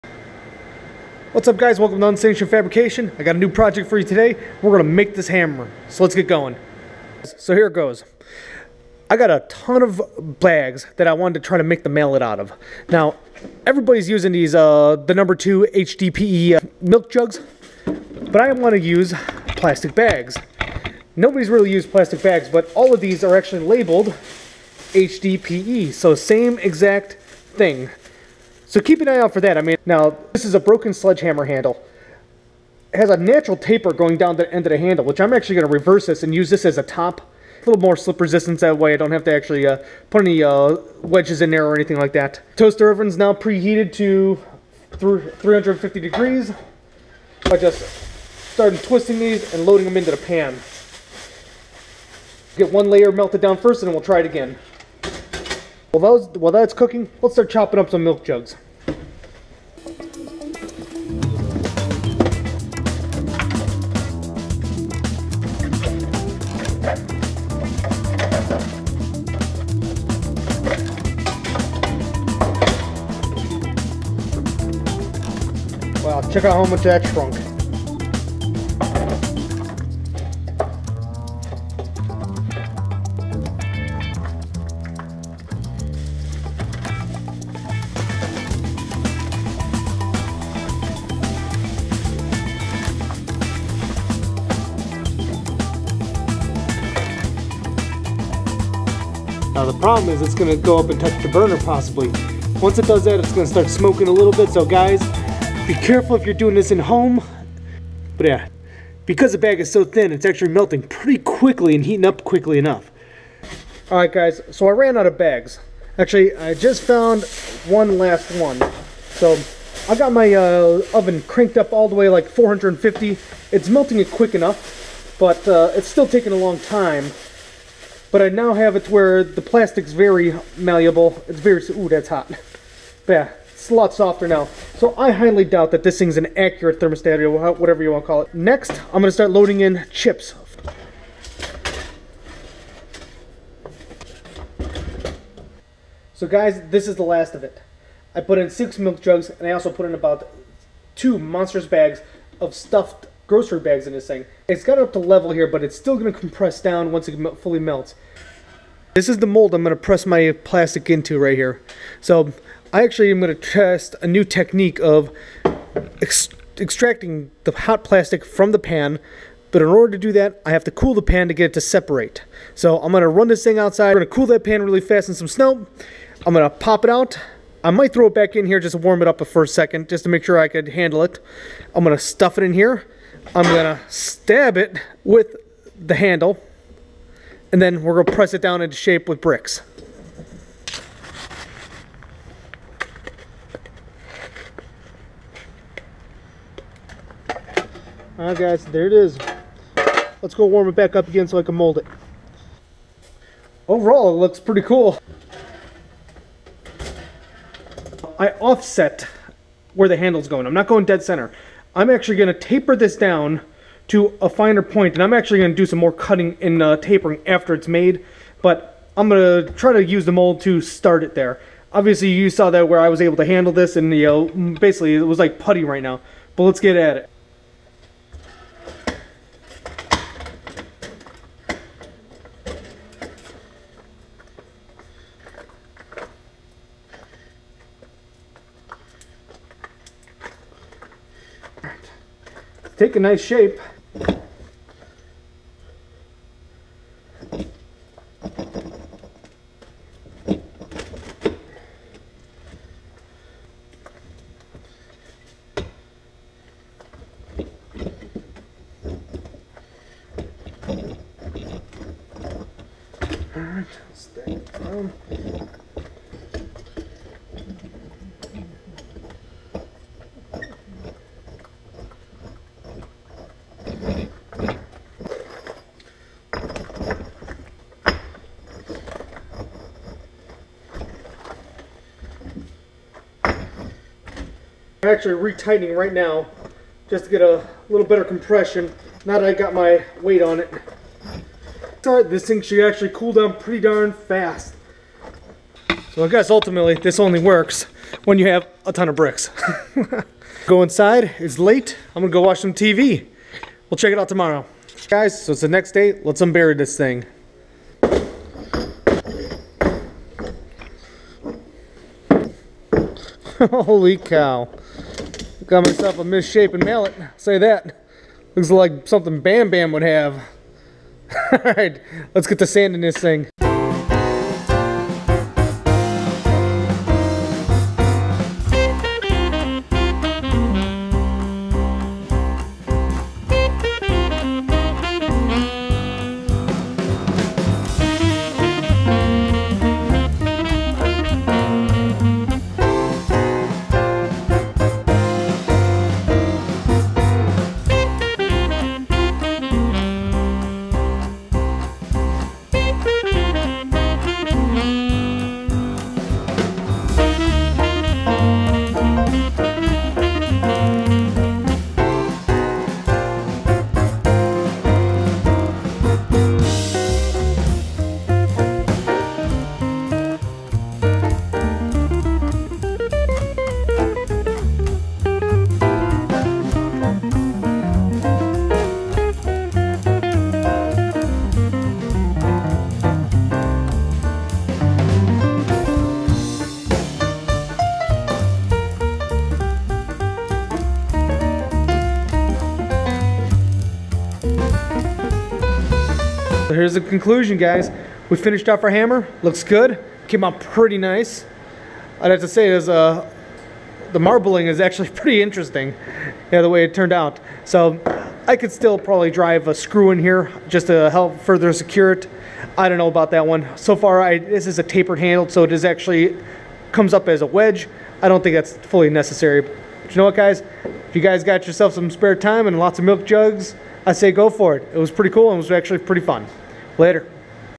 Recycled Bags & Jugs HDPE Mallet
6 milk jugs + 20 grocery bags + salvaged hammer handle + To Much time on my hands = Awesome hammer project!